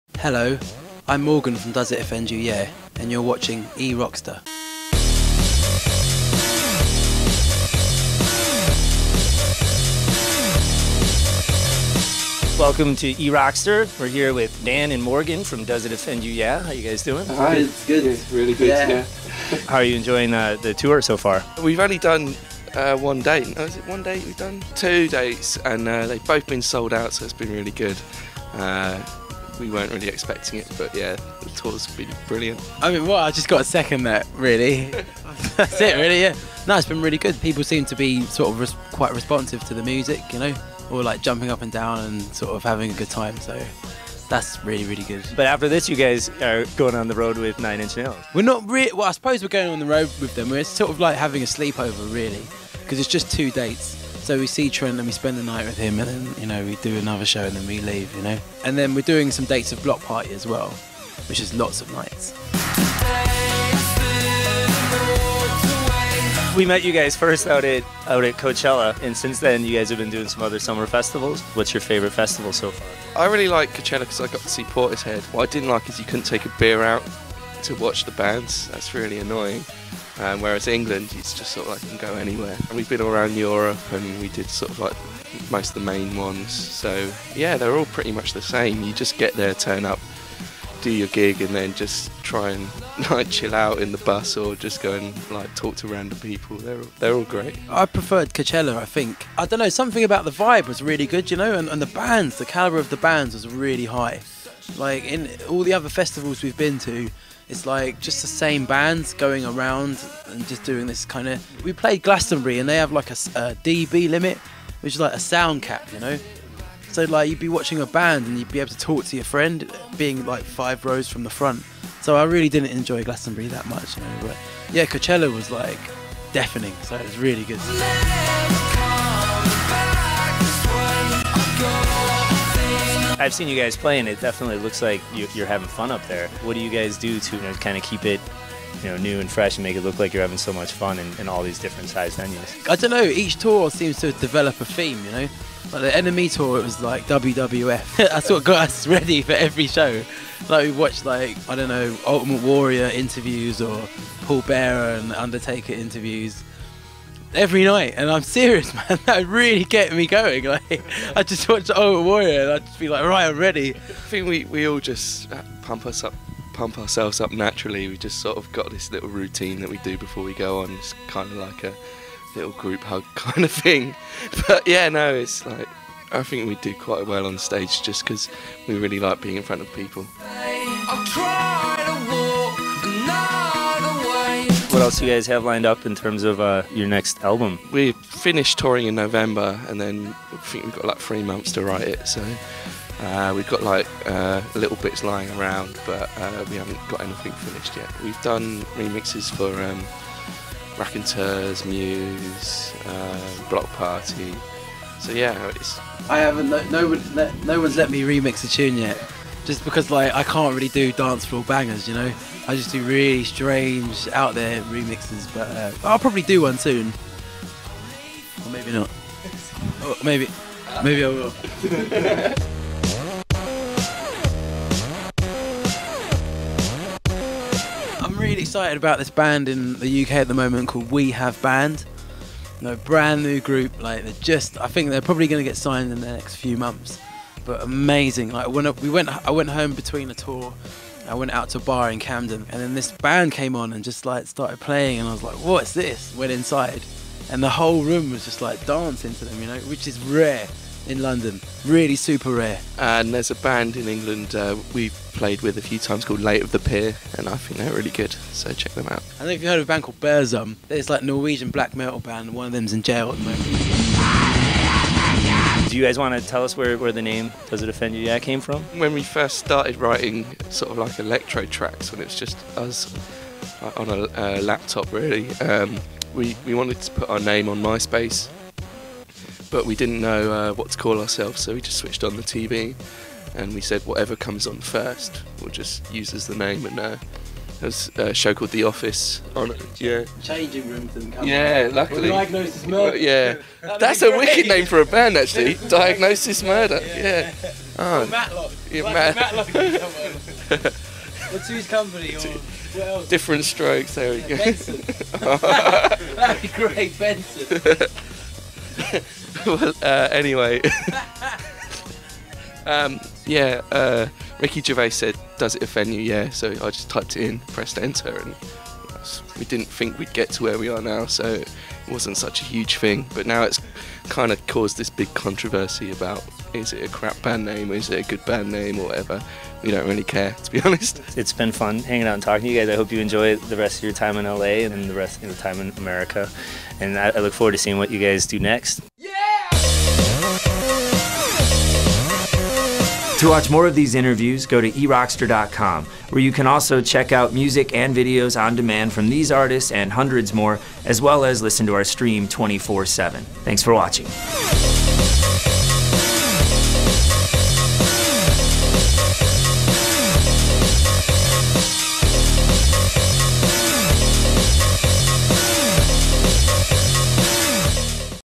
Does It Offend You, Yeah? interview with erockster.com
Eric Szmanda catches up with Morgan and Kevin to find out what's new since Coachella, their upcoming sleep over with Trent Reznor and Nine Inch Nails, and the liquor policies at outdoor festivals.